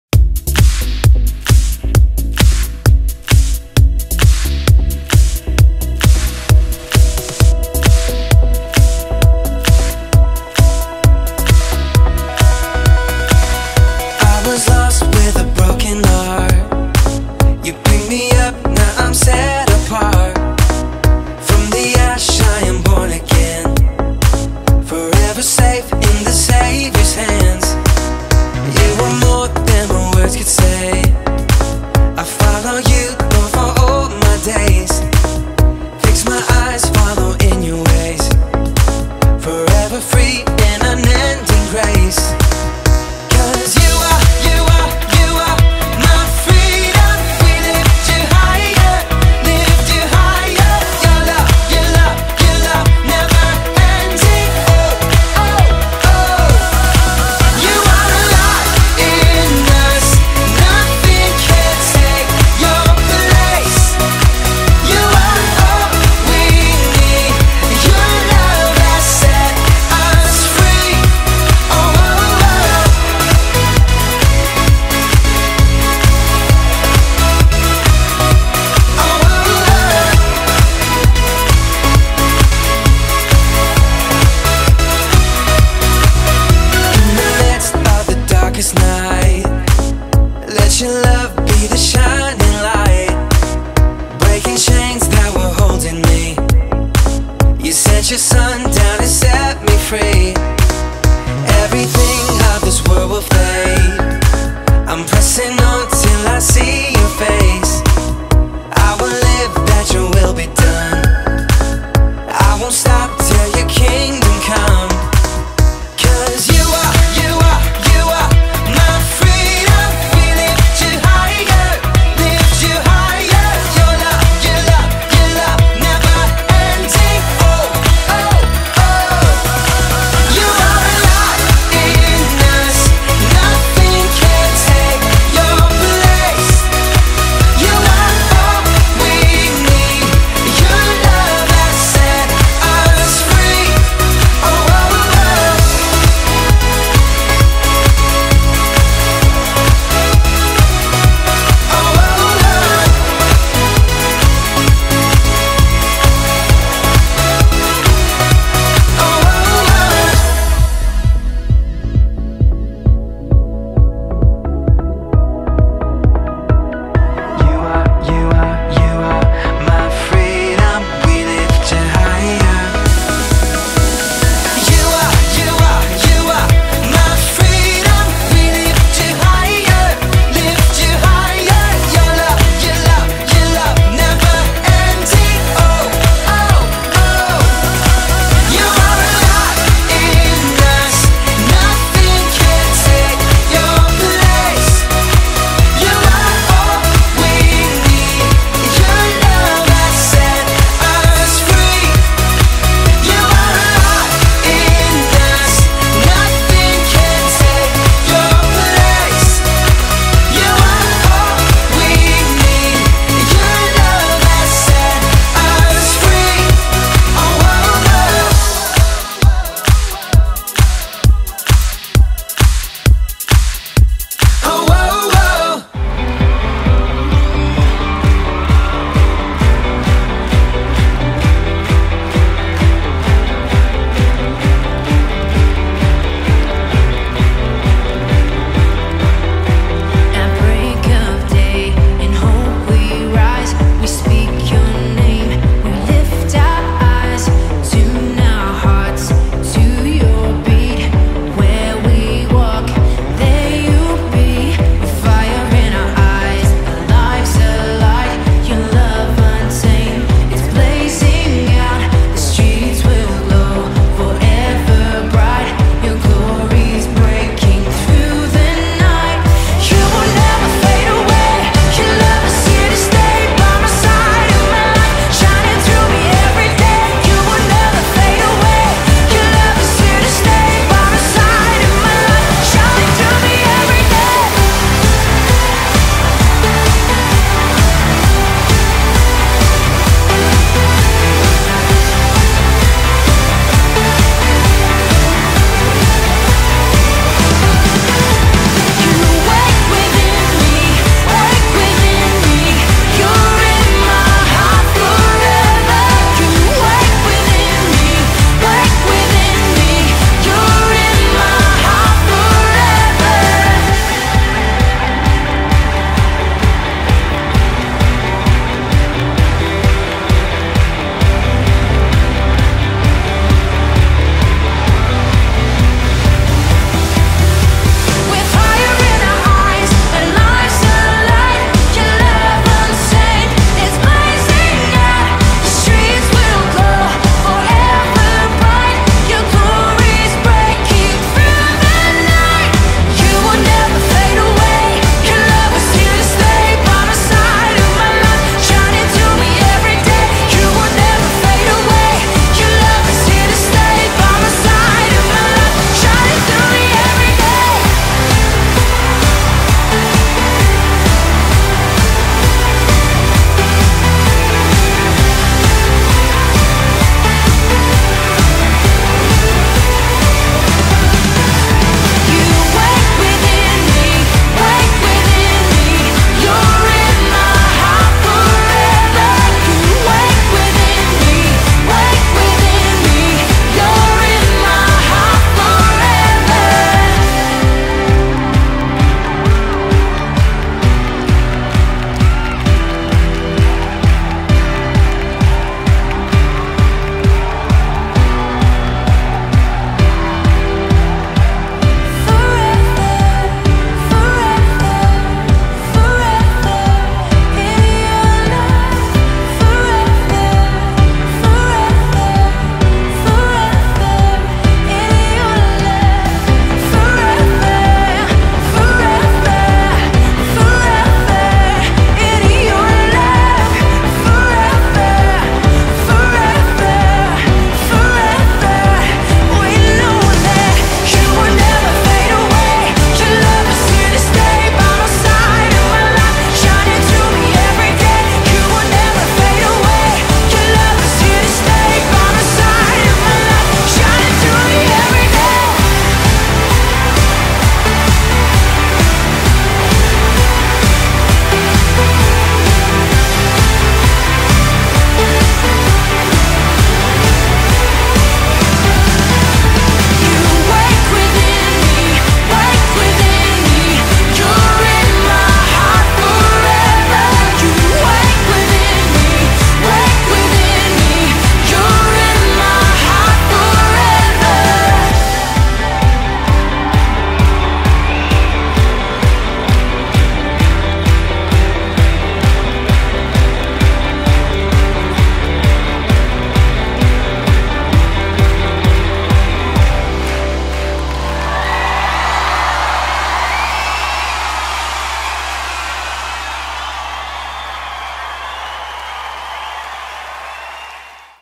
JUNIOR CAMP 2014: CALLED TO DUTY
Apostolic Assembly East Los Angeles District Junior Camp was an amazing experience for campers and staff alike. This year it was at Camp Arbolado in Angelus Oaks.